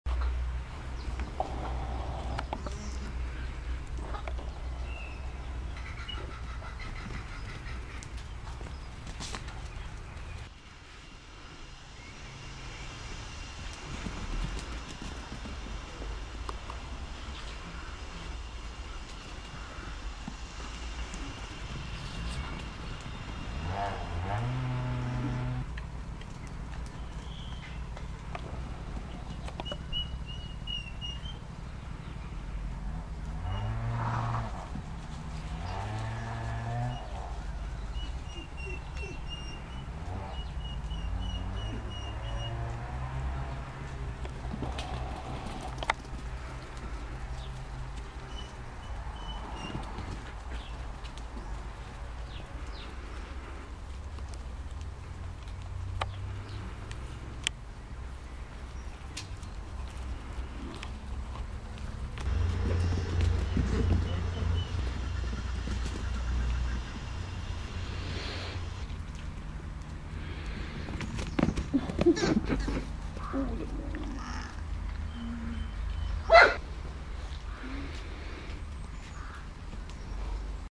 Alpaca cria's play a running and jumping game
Two alpaca cria's are running, jumping and falling down on the grass. A 2 weeks od female alpaca cria learns a 3 days old male alpaca cria how to use the meadow. At last she falls down and then they make a little break.